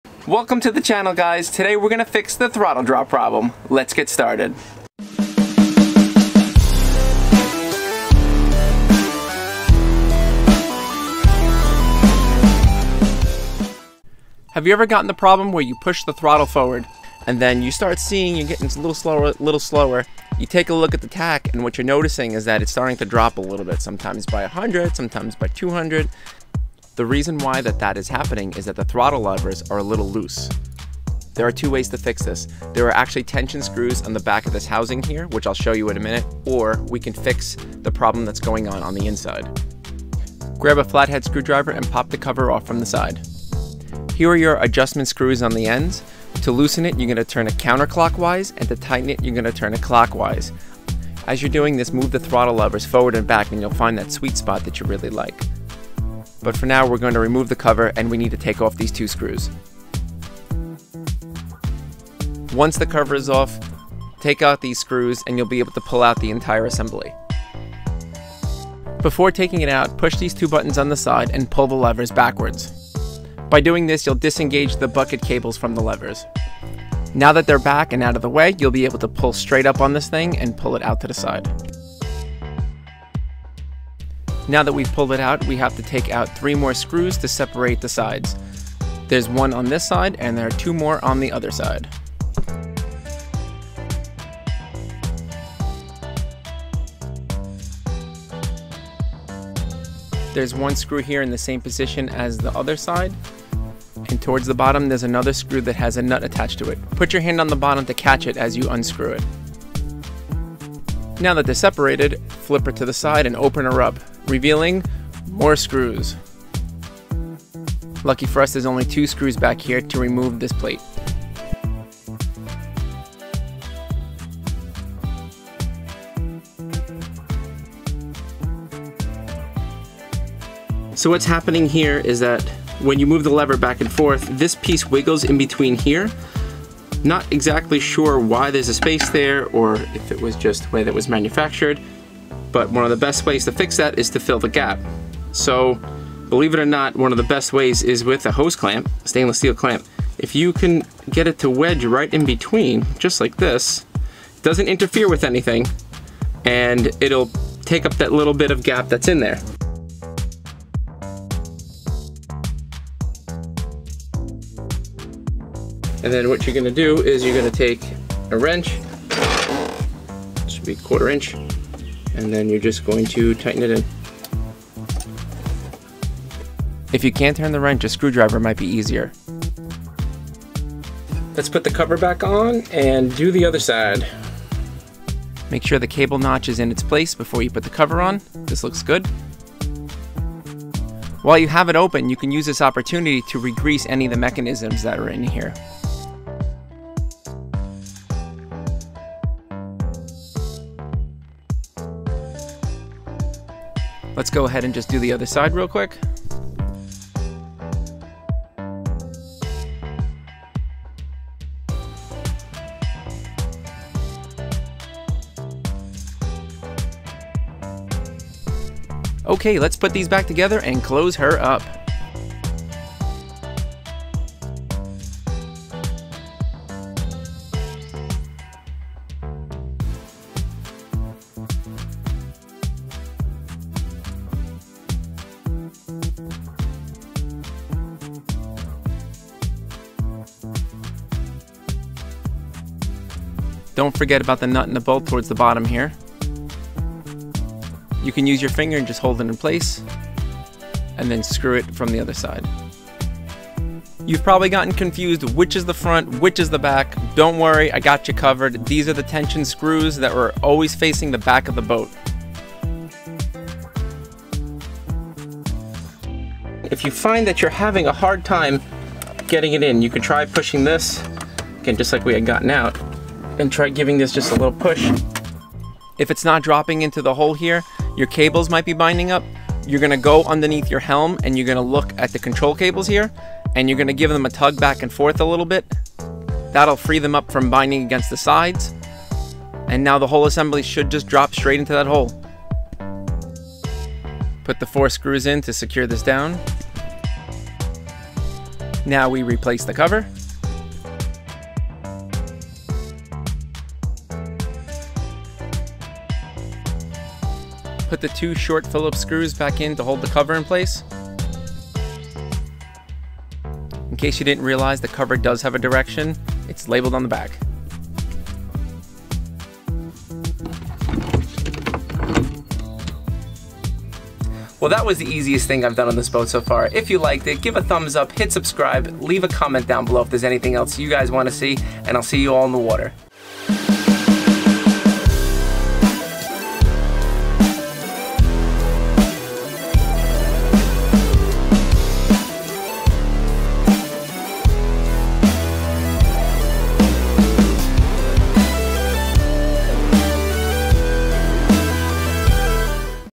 Yamaha Jetboat | Throttle Drop Fix
In this video we remove and dissemble the throttle housing on a Yamaha Jet Boat. We will perform a permanent fix to the rpm drop issue while underway. You usually have this issue on the older generation boats, before they switched to a drive-by-wire.

Chapters
00:00 Intro
00:40 Control Lever Assembly Removal
01:24 Disassembly
02:10 The Fix
03:01 Reassembly / Other Side Fix
04:13 Reinstall the Control Lever Assembly

2010 Yamaha Jetboat  SX240
Throttle Disassembly
Throttle Control Removal